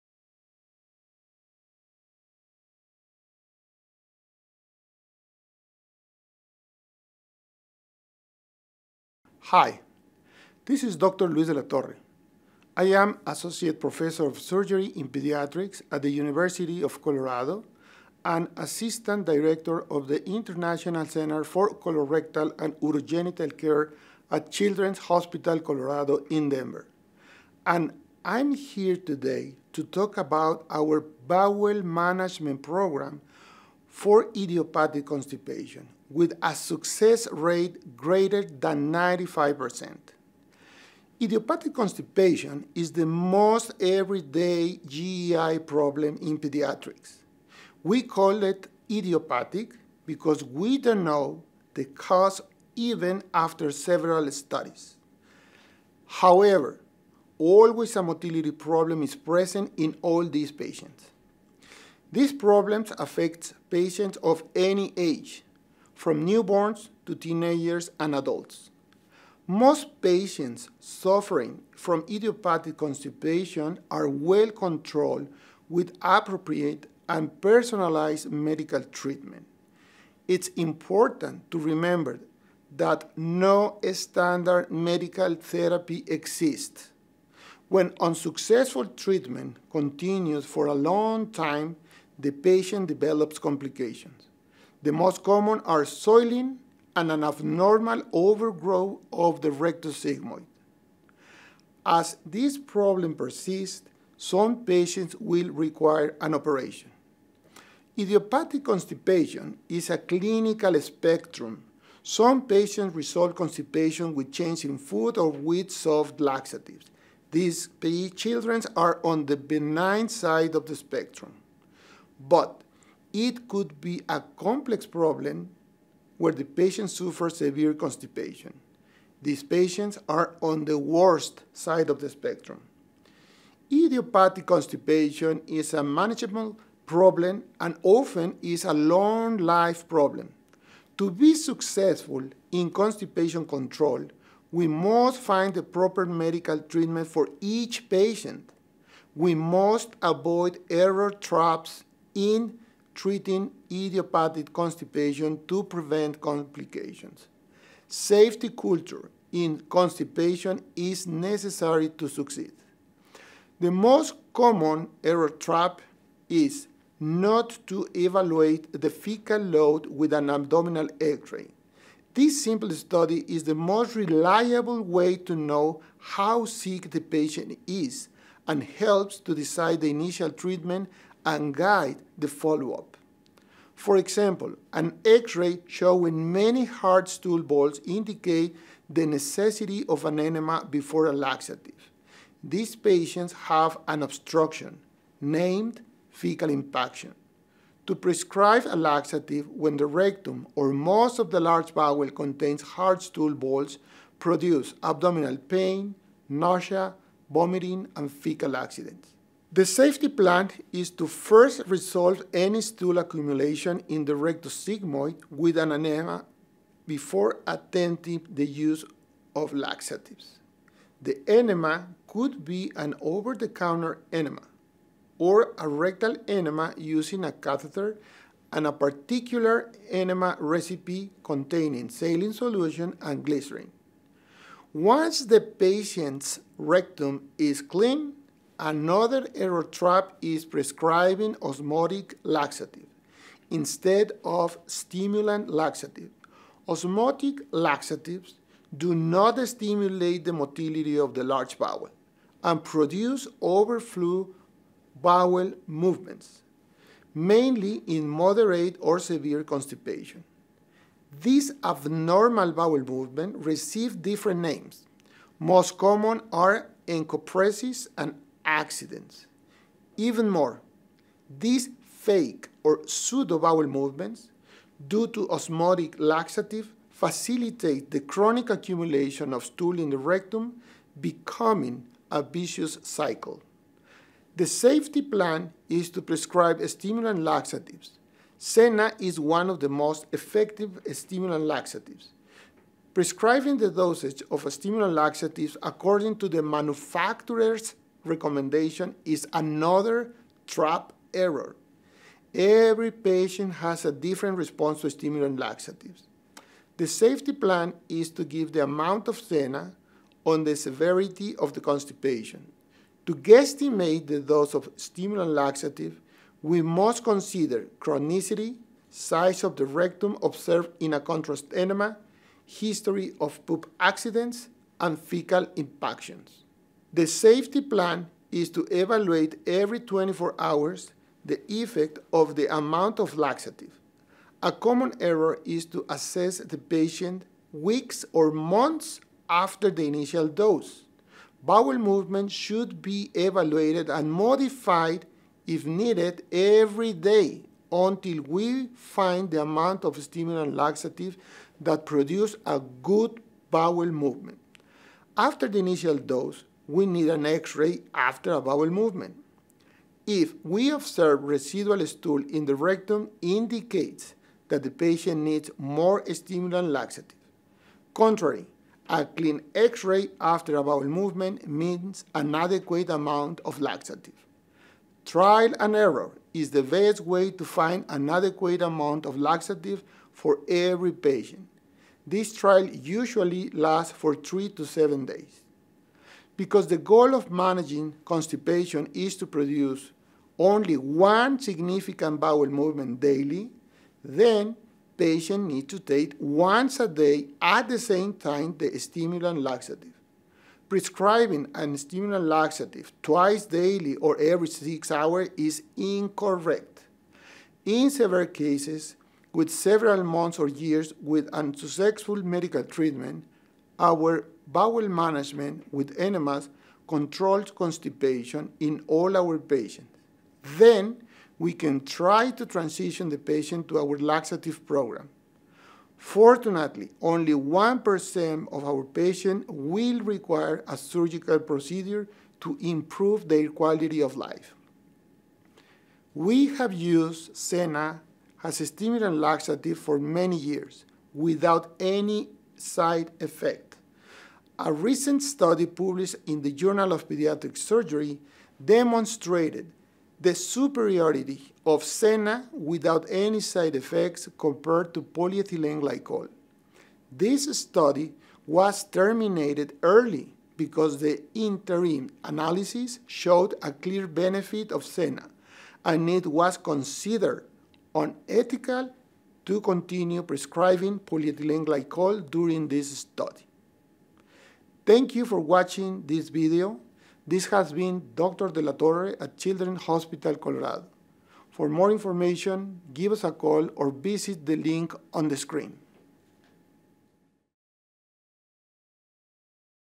Current Management of Idiopathic Constipation
Idiopathic constipation is one of the most common gastrointestinal problems in pediatrics, with a spectrum ranging from mild to severe. Although there’s no cure for idiopathic constipation, most patients do well with standard medical treatments like laxatives or changes in diet, with only 1% of severe cases requiring surgery.

In this video, Luis De la Torre, MD, provides an overview of bowel management at Children’s Hospital Colorado, which claims a success rate of over 95% for idiopathic constipation. Dr. De la Torre also describes the most reliable methods, such as abdominal X-rays, for assessing constipation severity and identifying initial treatment –– sometimes requiring enemas before starting on laxatives. As far as laxatives, Dr. De la Torre explains why senna is the superior option over polyethylene glycol.